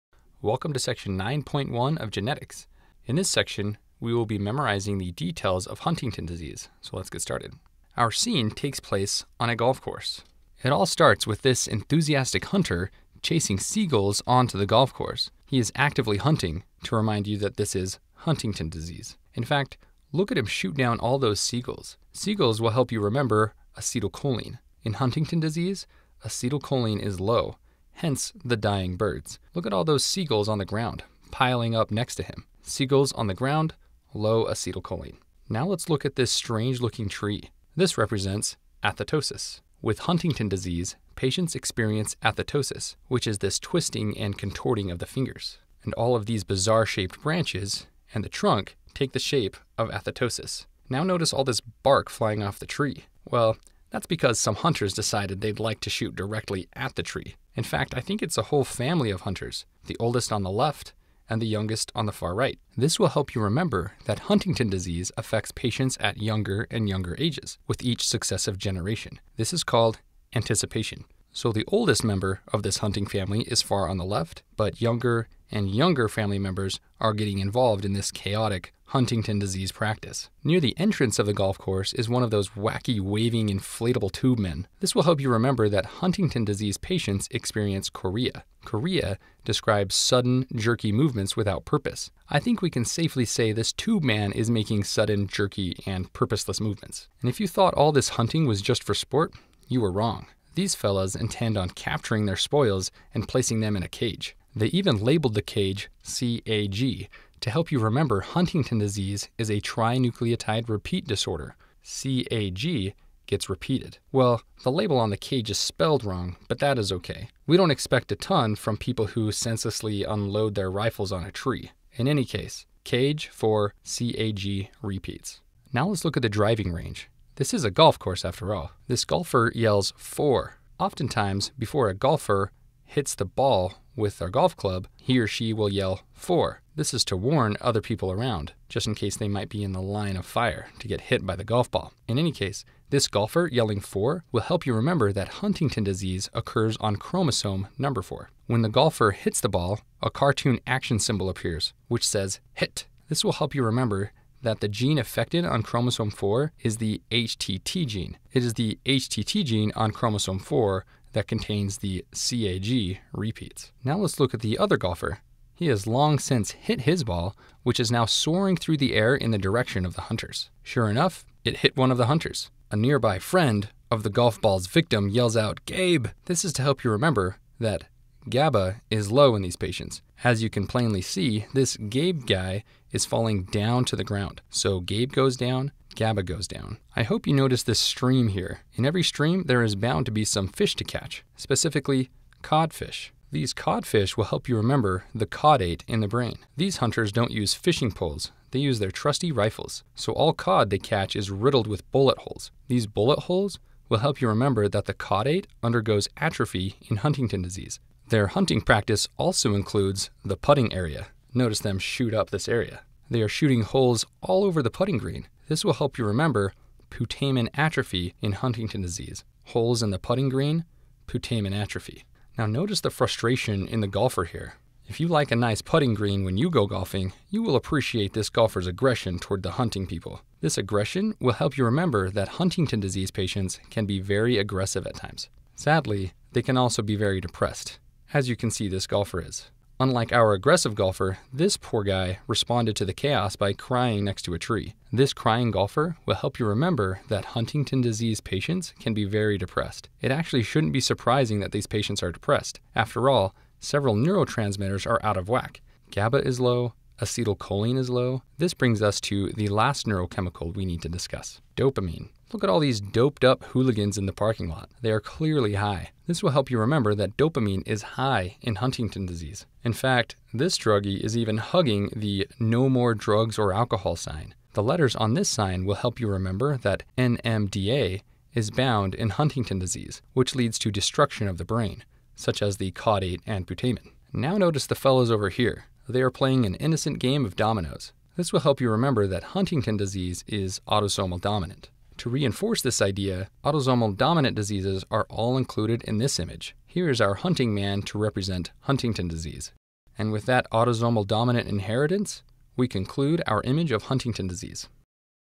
USMLE Step 1 Huntington Disease
Remember all the things about Huntington Disease for Step 1, forever. In this video you will fully understand (and remember) caudate atrophy, putamen atrophy, NMDA, chorea, CAG repeats, HTT gene on chromosome 4, low GABA and acetylcholine, high dopamine, anticipation, and athetosis.